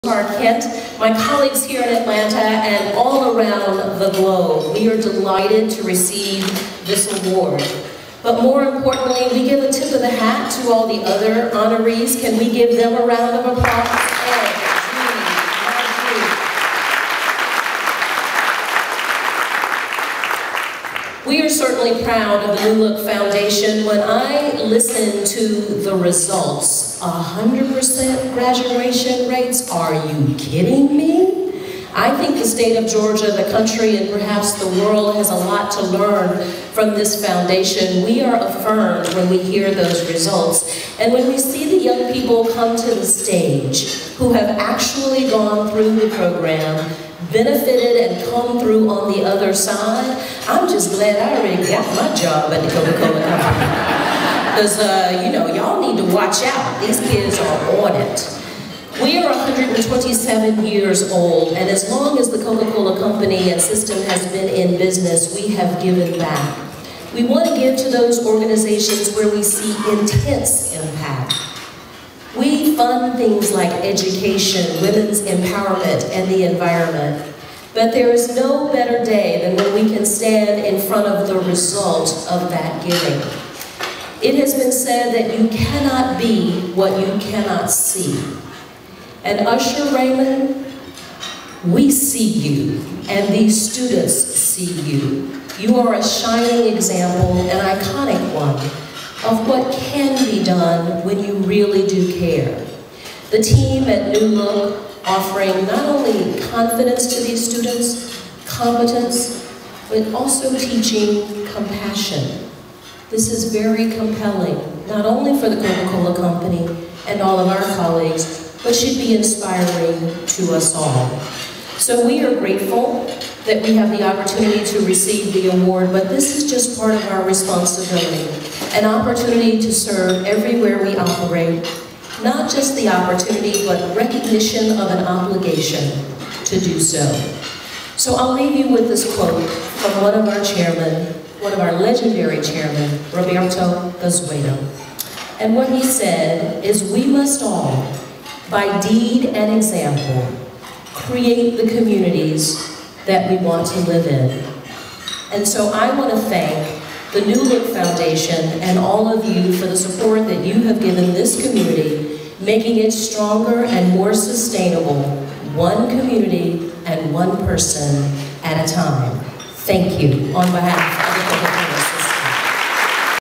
Lisa Borders, Coca-Cola, Accepts Service Legacy Award
Lisa Borders delivers a beautiful acceptance speech at this year's Presidents Circle Luncheon on behalf of the Coca-Cola Foundation. Listen to what being a part of New Look means to her.